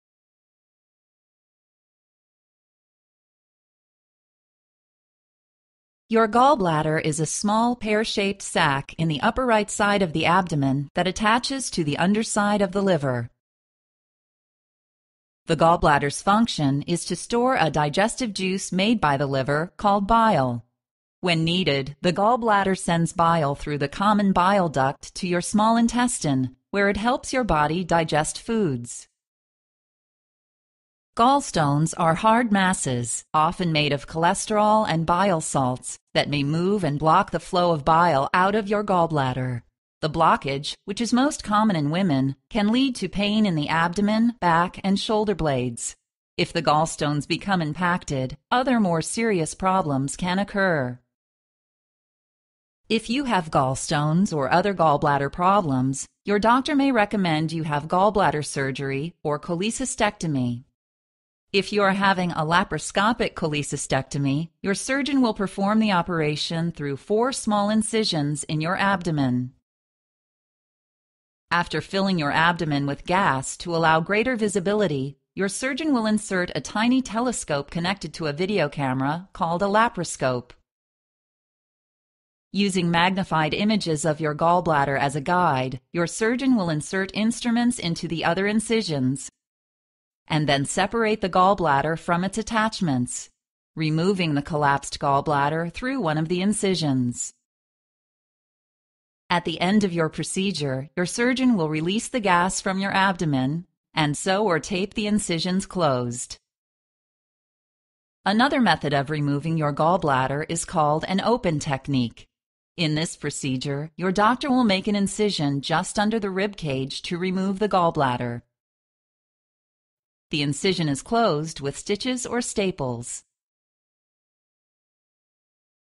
gallblader Removal day surgery animation vedio
This medical animation shows laparoscopically assisted gallbladder removal surgery, or cholecystectomy. The animation begins by showing the normal anatomy of the liver and gallbladder. Over time, gallstones form within the gallbladder, blocking the cystic duct, and causing the gallbladder to become enlarged and inflamed. The procedure, sometimes called a "lap-chole", begins with the insertion of four trocar devices, which allow the physician to see inside the abdomen without making a large incision. Air is added to the abdominal cavity to make it easier to see the gall bladder. Next, we see a view through the laparascope, showing two surgical instruments grasping the gallbladder while a third severs the cystic duct. After the gallbladder is removed, the camera pans around to show that the cystic artery and vein, have already been clipped to prevent bleeding.